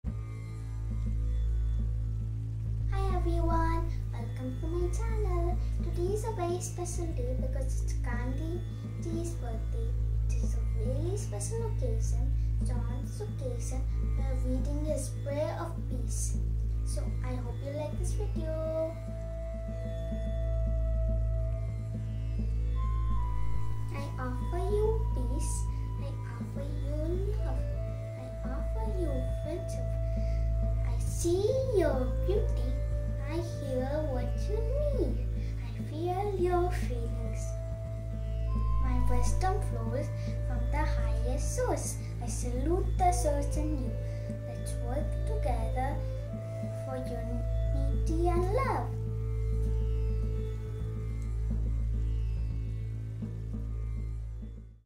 Gandhi Jayanti Poem | Gandhi Jayanti Prayer | Gandhi Jayanti Speech | Gandhi Jayanti ka gana | Anvi
GANDHI’S PRAYER FOR PEACE Recitation by Anvi
GANDHI’S PRAYER FOR PEACE
I offer you peace
I offer you love
I offer you friendship
I see your beauty
I hear your need
I feel your feelings
My wisdom flows from the highest source
I salute that source in you
Let us work together
For unity and peace.

🌸❤️GIVE this video a BIG Thumps Up🌸❤️
🌸❤️WRITE Lovely comment you all lovely people🌸❤️

LOVE❤️❤️
Anvi